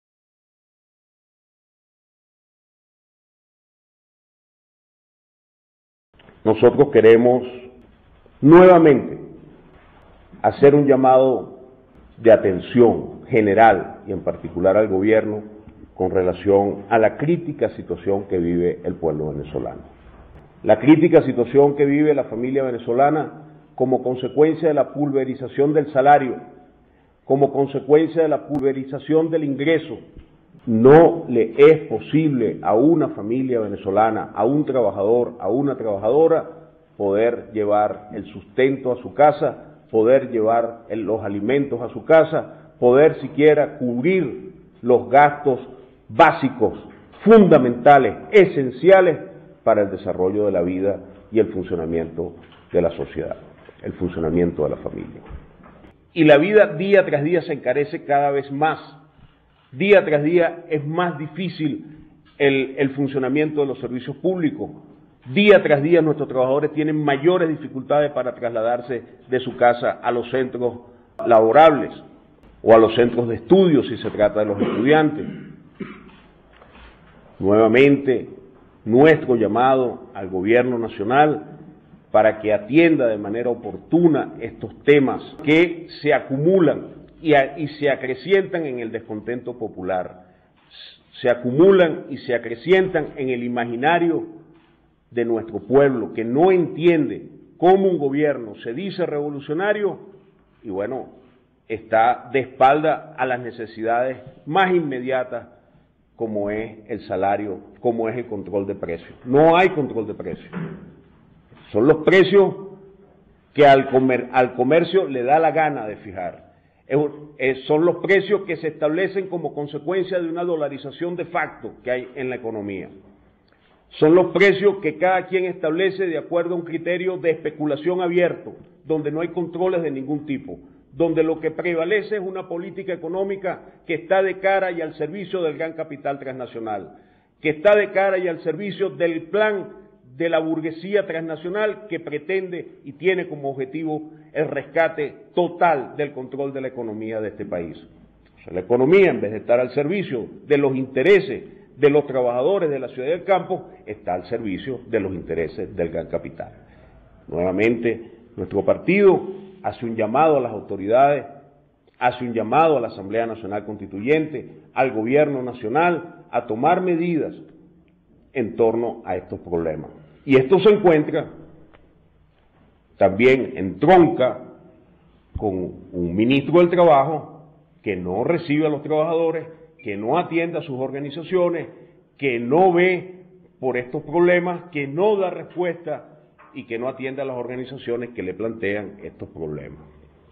PCV: Gobierno y ANC deben tomar medidas ante crítica situación económica del pueblo
5-Ago-2019
Vocero: Carlos Ojeda Falcón, miembro del Buró Político del Partido Comunista de Venezuela (PCV)